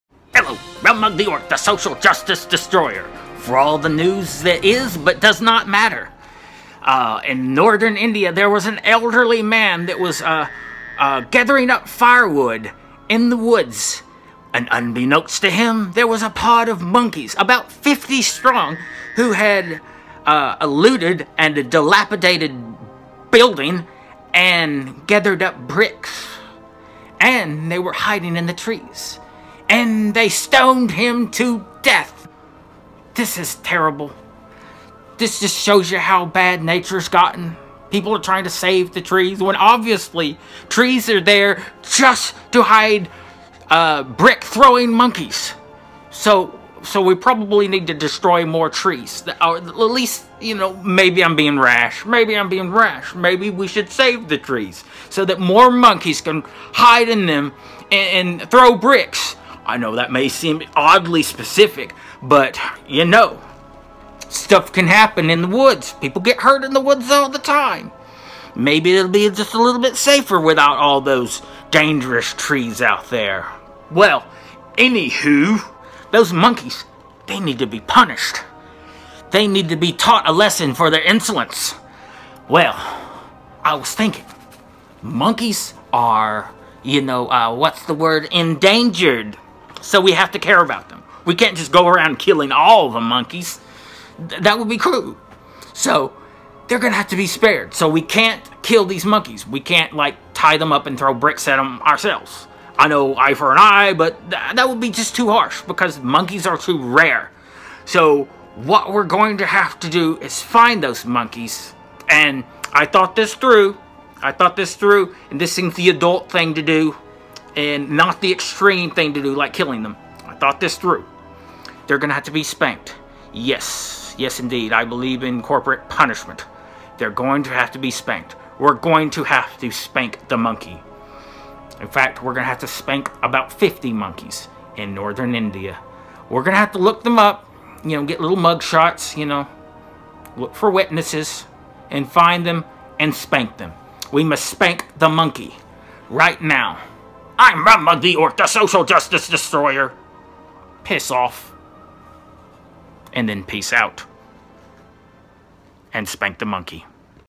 Insane Monkeys, Stoned Man To Death!!!!!
Rum Mug the Orc has heard about a terrible event, where a man sadly lost his life, because of unruly monkeys. Rum Mug fears this could happen again, so he has a solution!!!